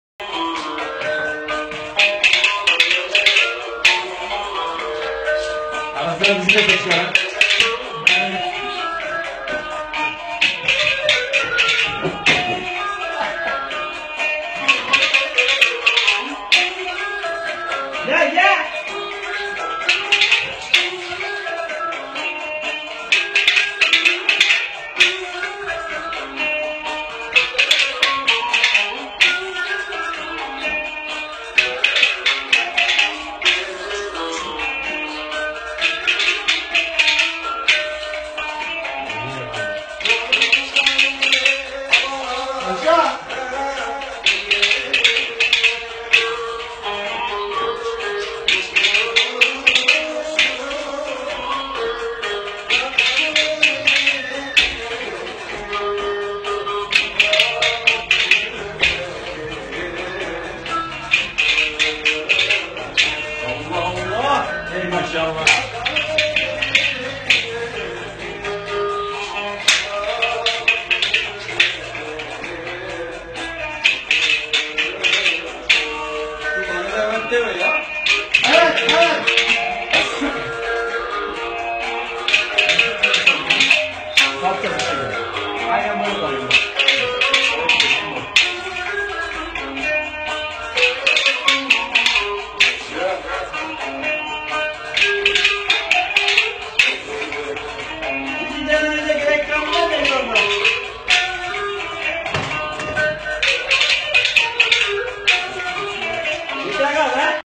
islam oğlu
berber dükanında amca islam oğlu oynuyor